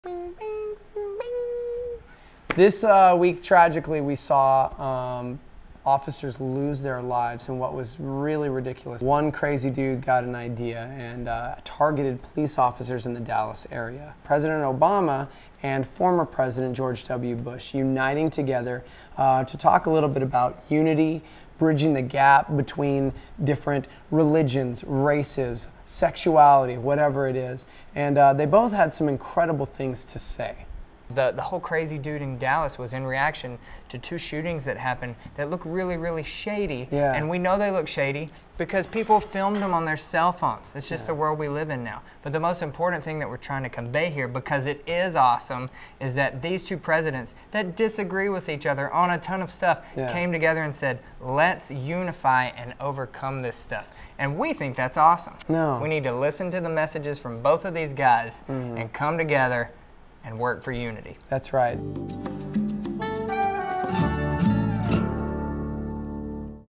THE MORE YOU AWESOME: Unity
President Obama and former president George W. Bush both had some amazing things to say about unity during the July 12th memorial, but social media took the whole event into the weeds, seemingly missing the point. Let's ignore the Spin, and really listen to what they each had to say. Unity and Compassion. Because finding the Awesome amidst so much suffering and anger is what will get us through it.